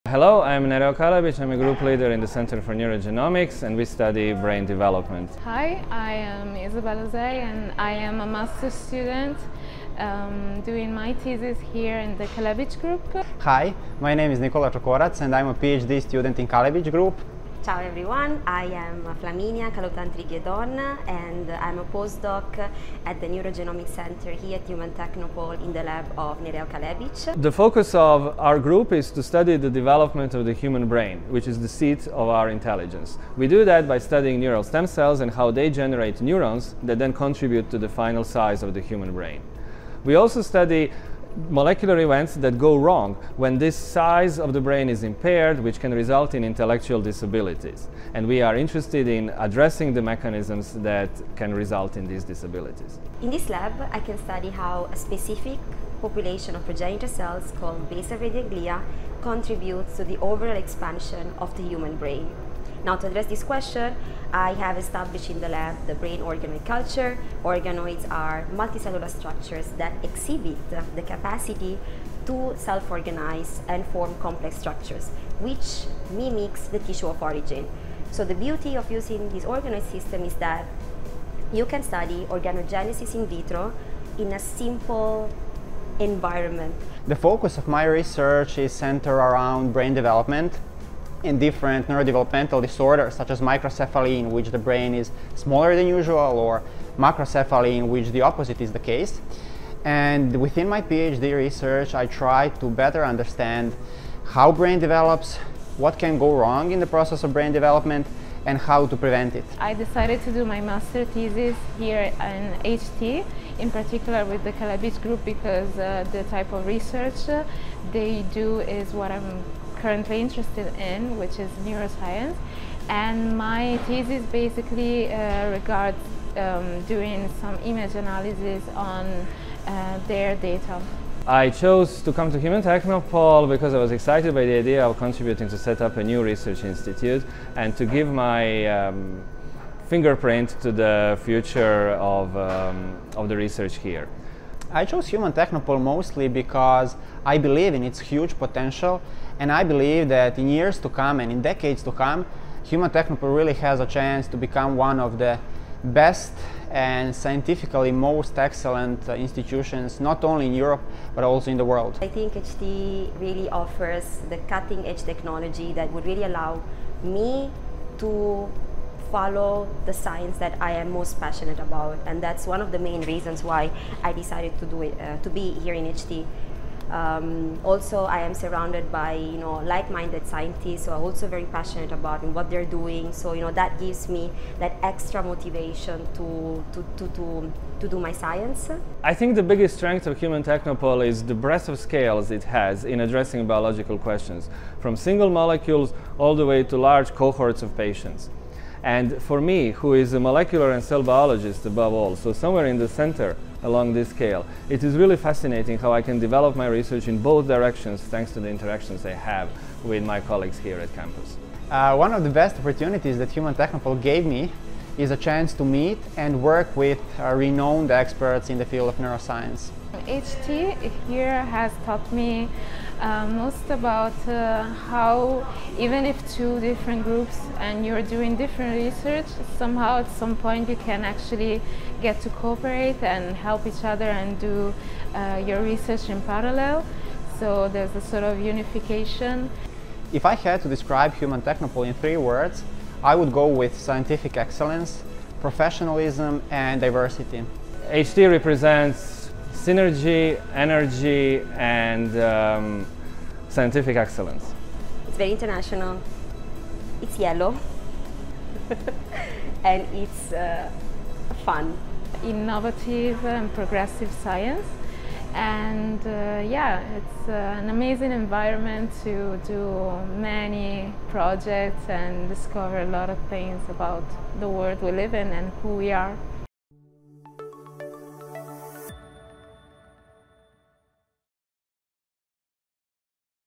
Meet the Kalebic Group - Neurogenomics Research Centre
How does the human brain develop and what can go wrong in the process? The Kalebic Group (Human Technopole, Neurogenomics Centre) focuses on the molecular and cellular biological mechanisms underlying human neocortex development and its implications for human evolution and neurodevelopmental disorders.

Meet Nereo Kalebic (Group Leader), Flaminia Kaluthantrige Don (Postdoc), Nikola Cokorac (PhD student) and Isabella Osei (Master Student).